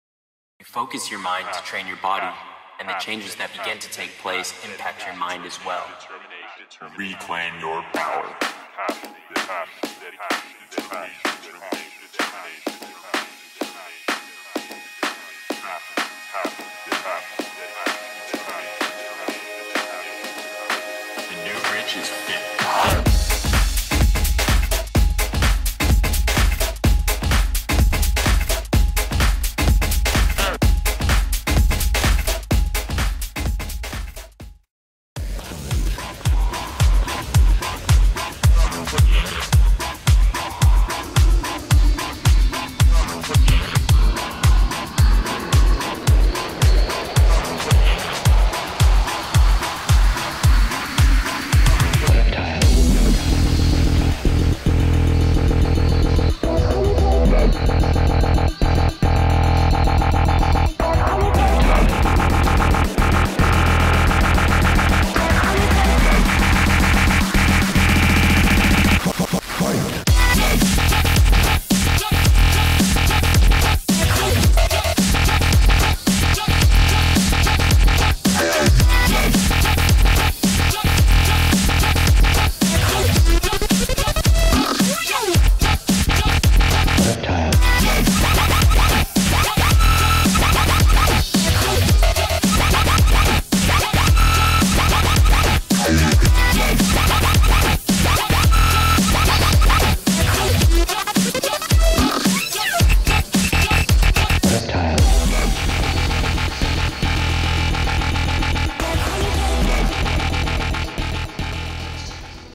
Body Weight Workout (Get Ripped Quick) [Build Muscle Fast]
Use this routine to build muscle and burn fat fast! Als to help you get a 6 pack quick!fat